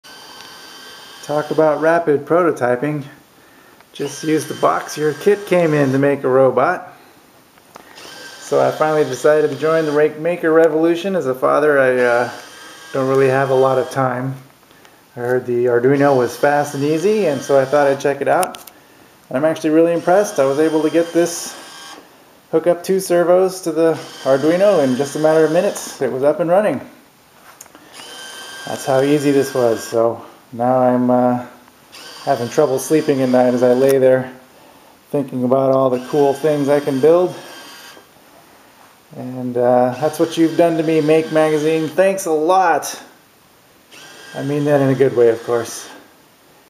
Make Box Bot
Quick bot build using the Arduino Kit from Make.